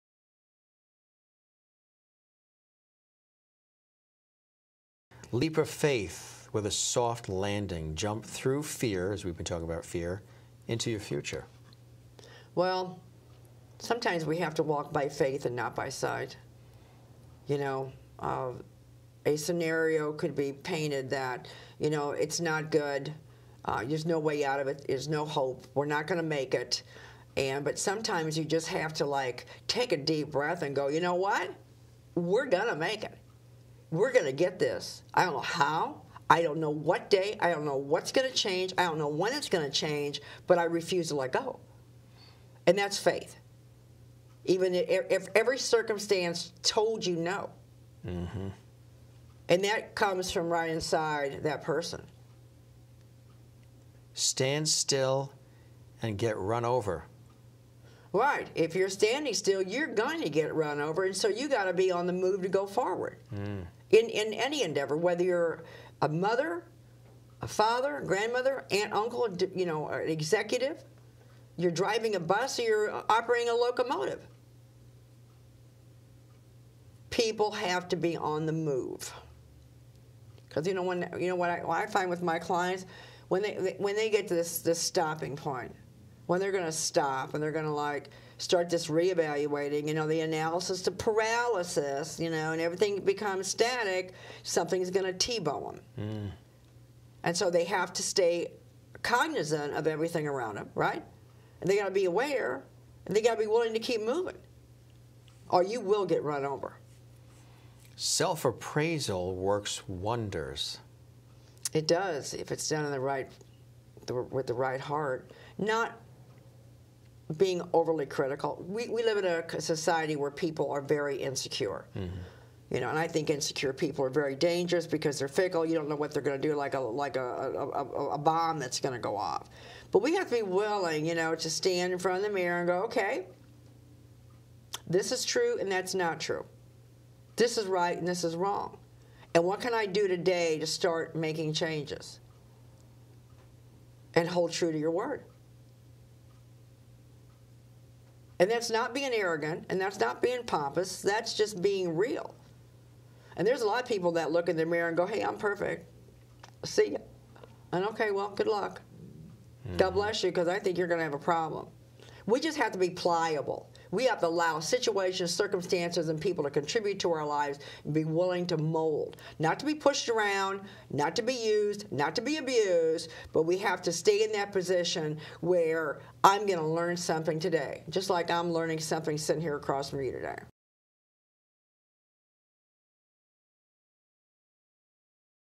Debra Rothschild - Jumping Through Fear
Jim Masters of Close-Up Television News talks with life coach Debra Rothschild.